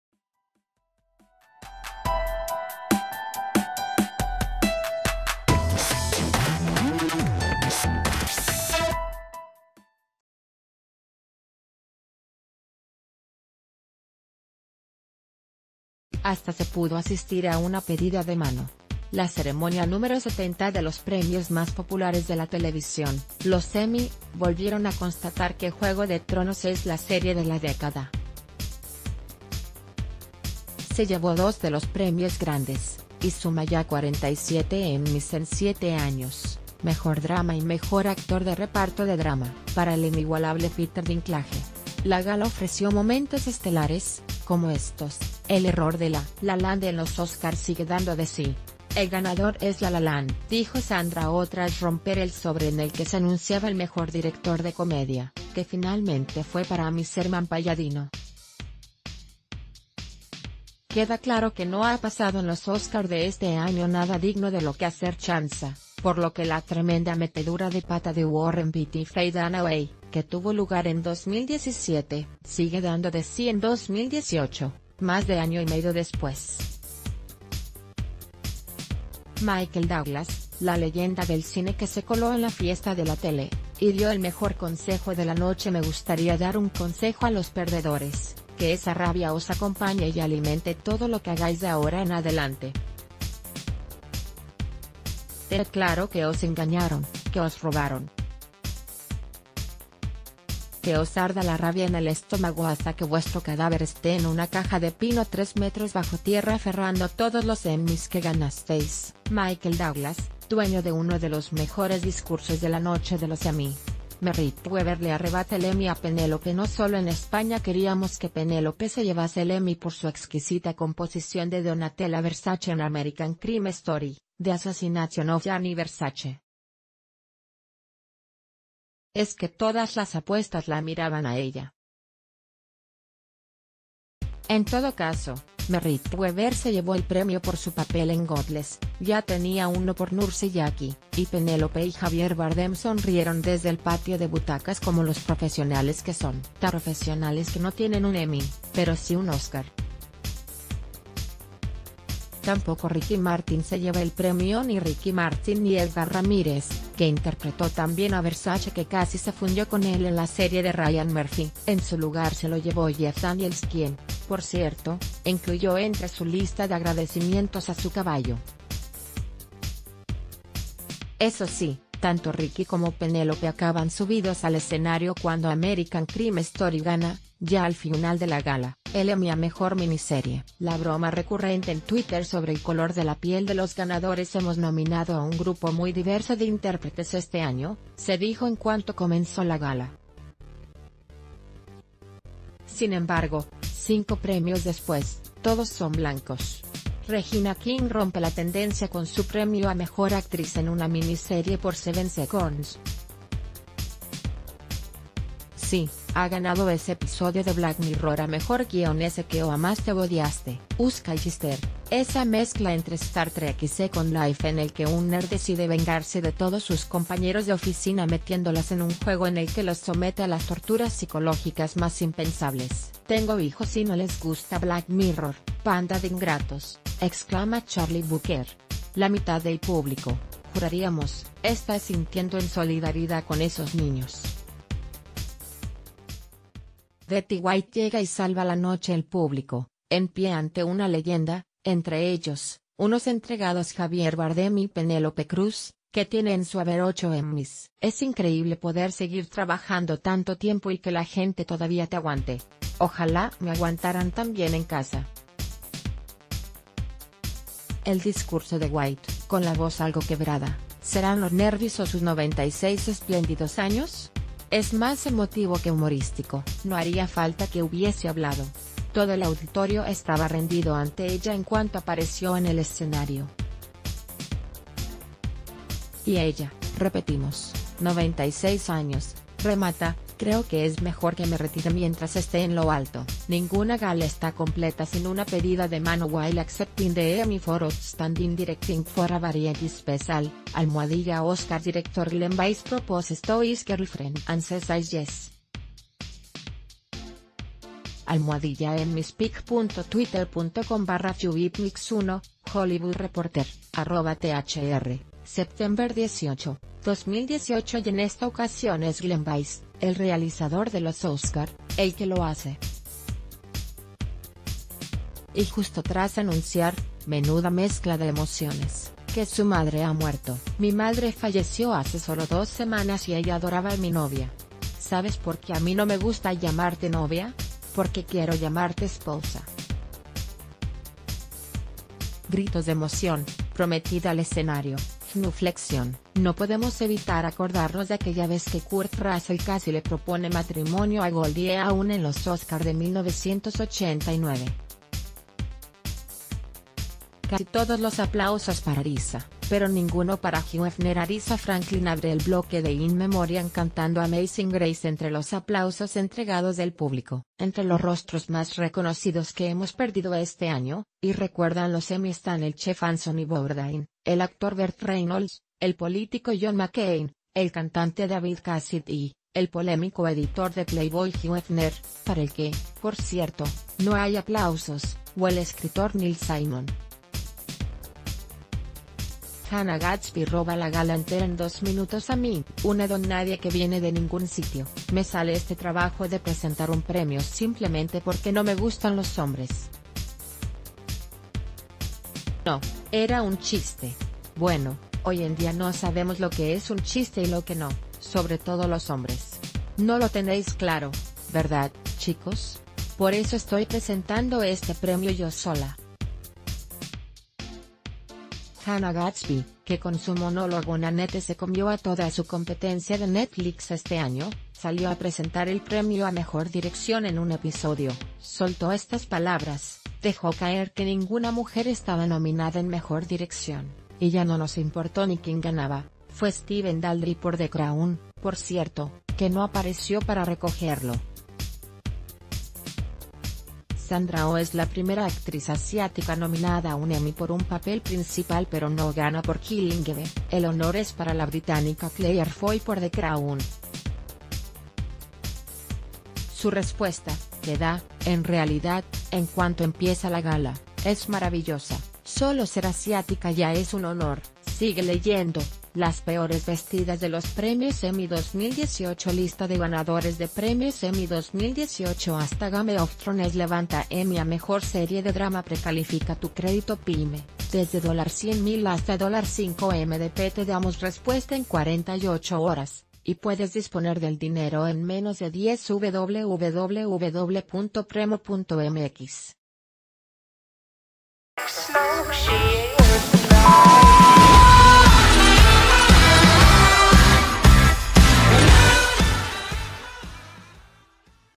Repasa los mejores momentos de los premios Emmy 2018
Hasta se pudo asistir a una pedida de mano. La ceremonia número 70 de los premios más populares de la televisión, los Emmy, volvieron a constatar que Juego de tronos es la serie de la década. Se llevó dos de los premios grandes (y suma ya 47 emmys en siete años): Mejor Drama y Mejor Actor de Reparto...